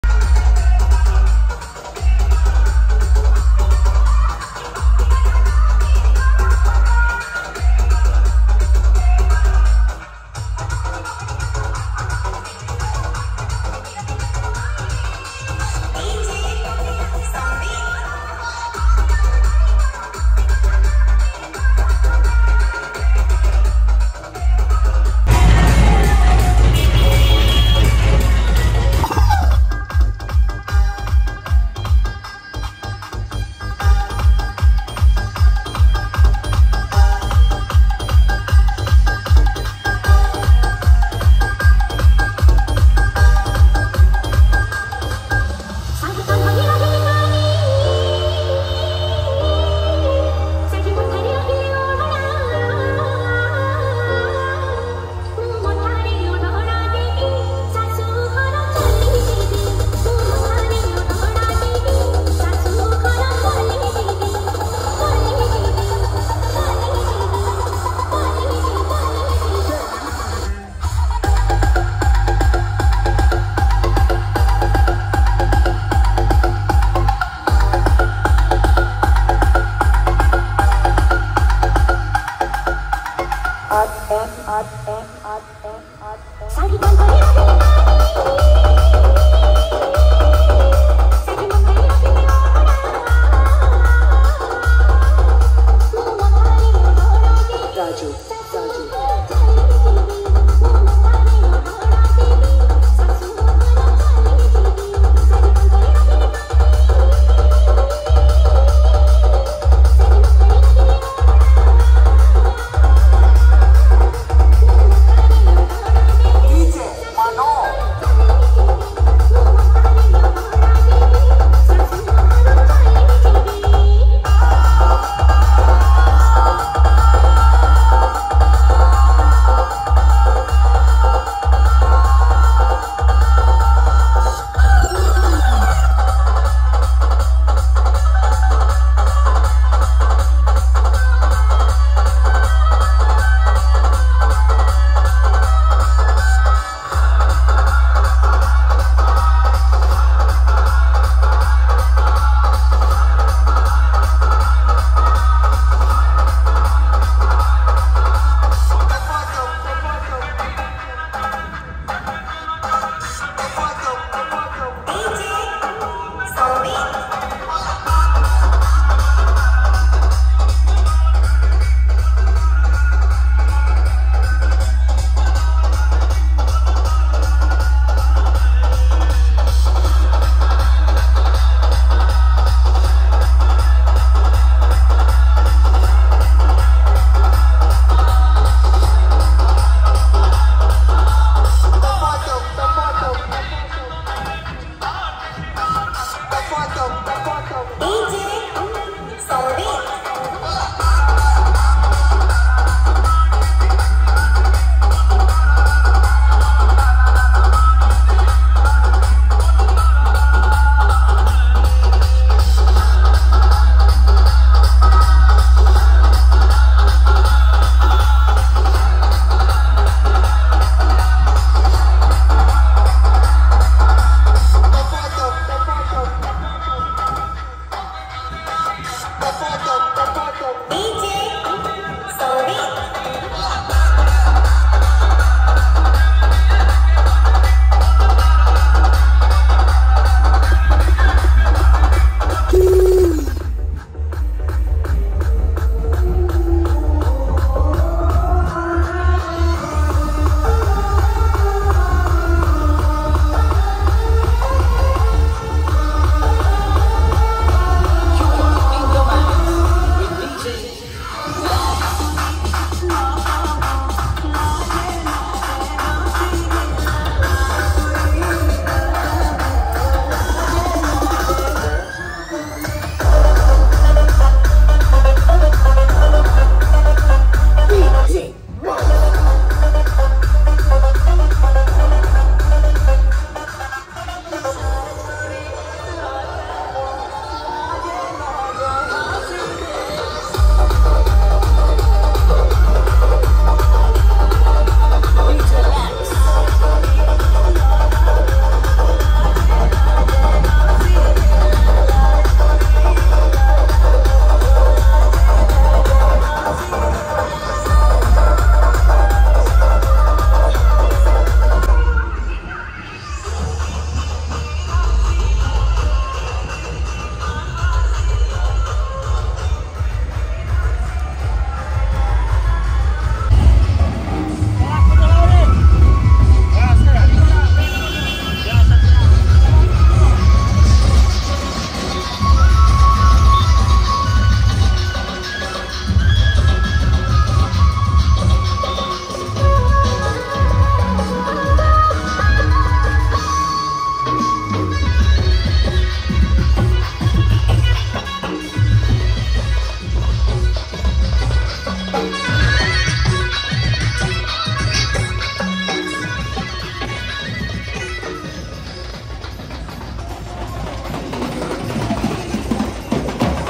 Dj HiTech New Setup 2023 Night Marriage Program Angul Market | Odisha Music Event
Visit Again.